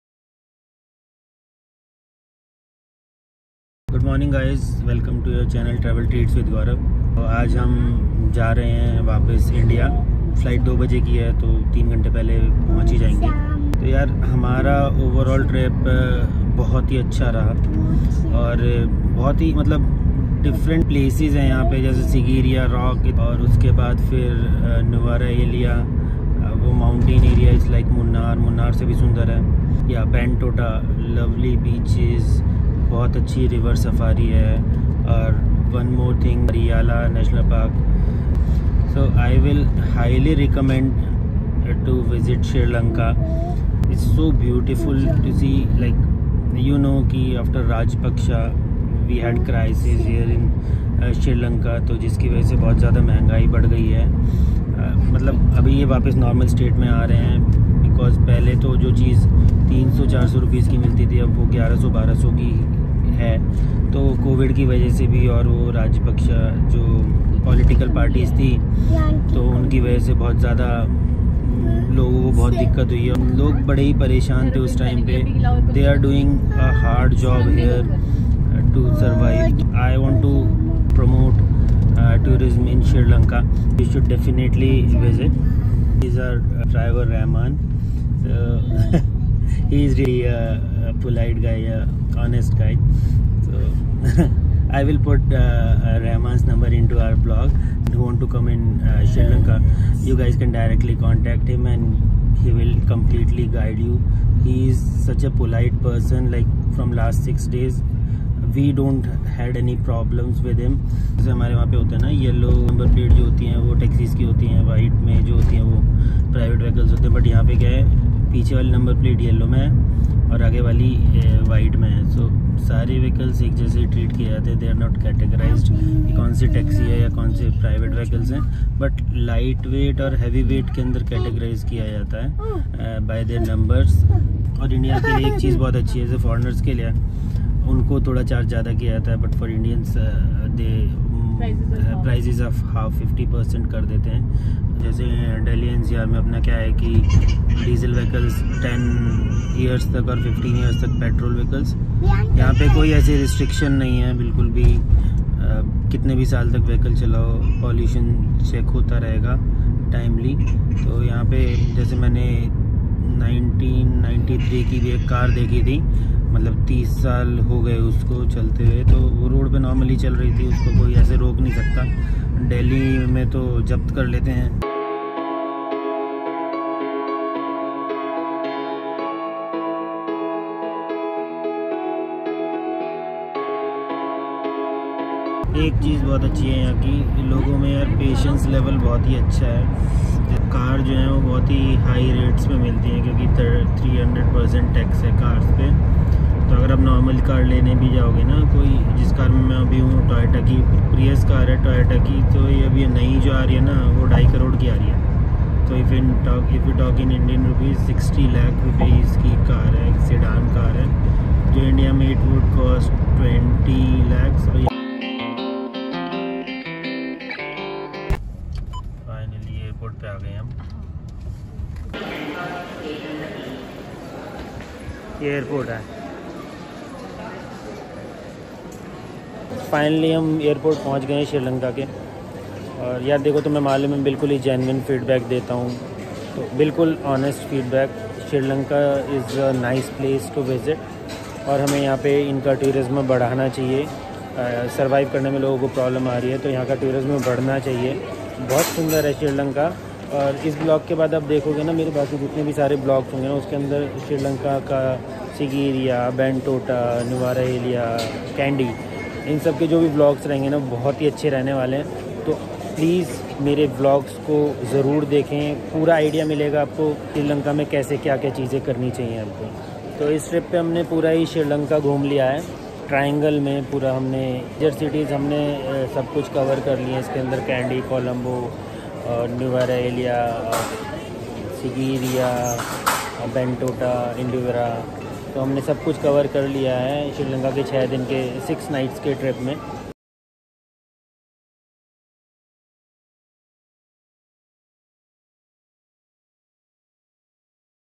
Sri Lanka - Heart of the Indian Ocean | Visit Sri Lanka | Promoting Tourism In Srilanka
For Complete Information about Sri Lanka Packages, You can contact Rehman - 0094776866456

I visited Sri Lanka in December 2023.
Sri Lanka may be a small island in the Indian Ocean but that’s the only thing small about it. The country, formerly known as Ceylon, boasts an ancient civilization, golden sandy beaches with their swaying coconut palms, mountains, and tea plantations
From the monumental views of Sigiriya Rock to the rolling tee fields of Nuwara Eliya, Sri Lanka left me speechless every day.